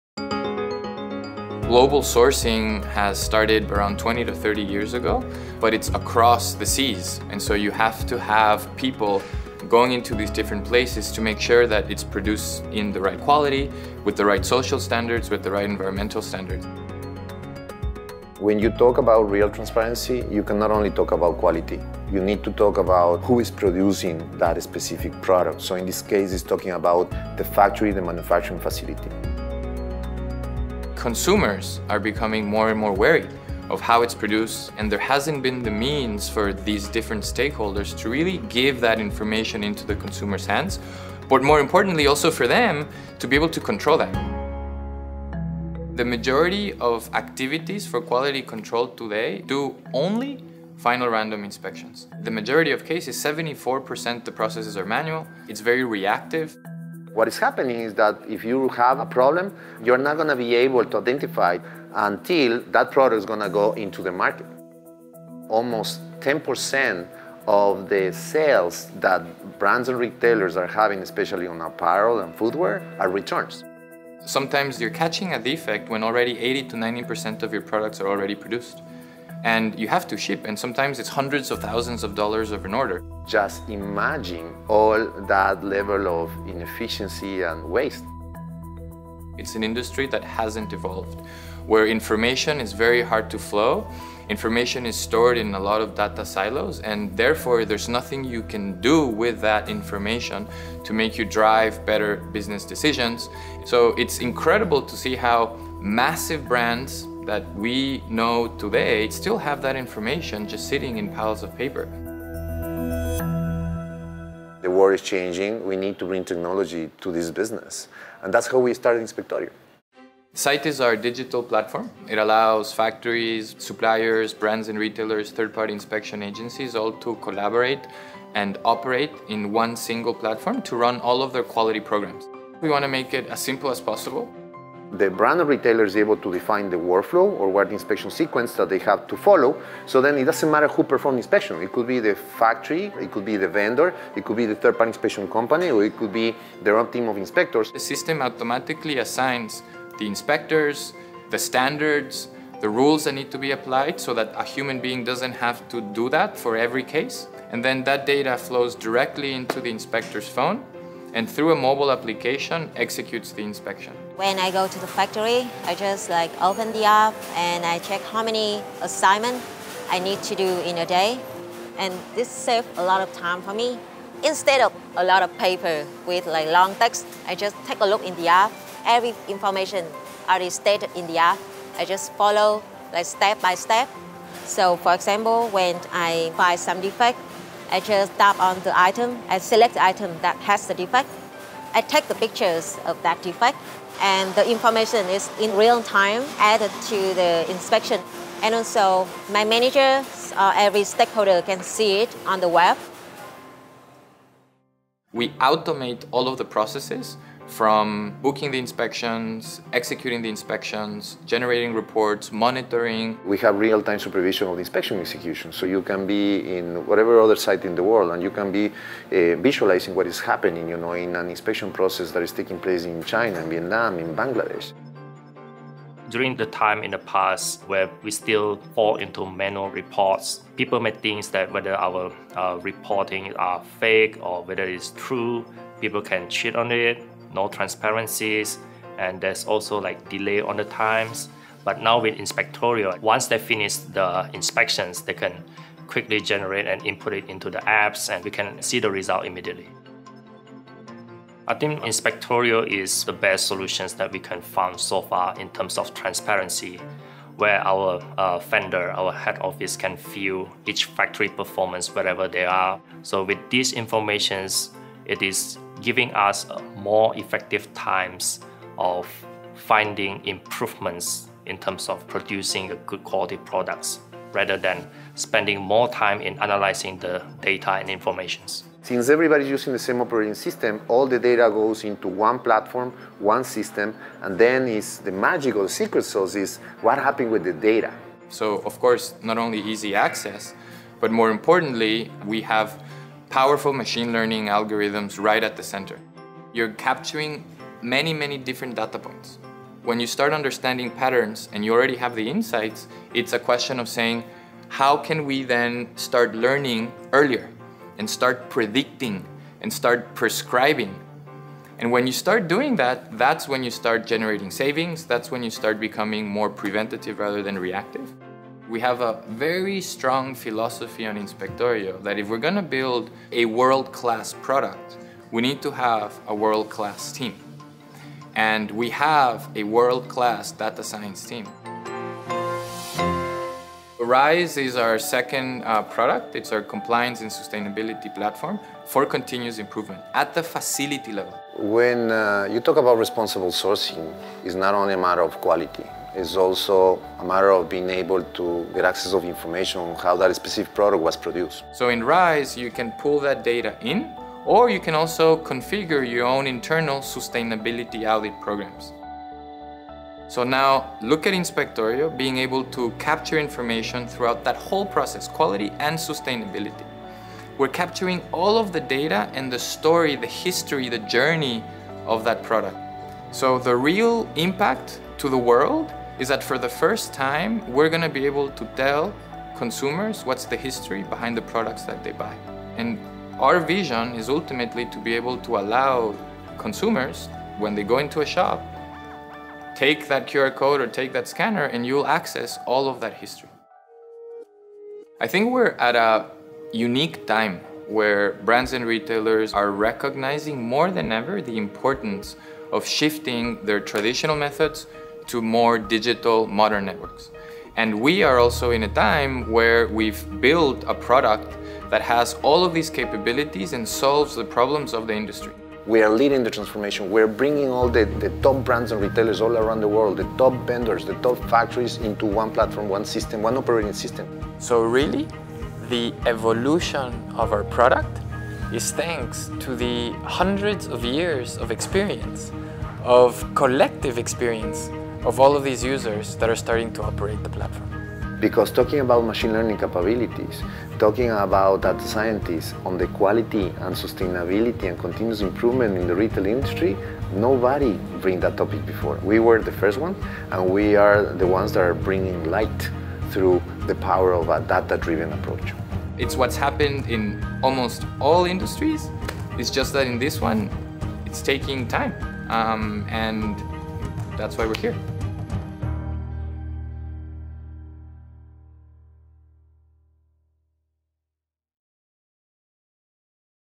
Inspectorio: The Best Software Platform for Quality and Compliance Monitoring
Business Reporter finds that 80-90% of products are produced before the quality has been verified, leading to hundreds of thousands of dollars wasted. What if you could improve quality while reducing costs?